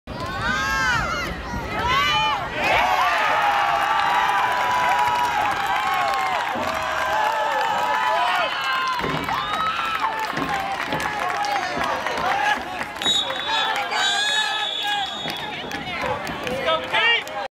Forbes MS Soccer
GOALLLLL! Forbes MS Soccer vs Benold. Last goal of the season!!! Way to go team! Forbes Falcons! May 18, 2015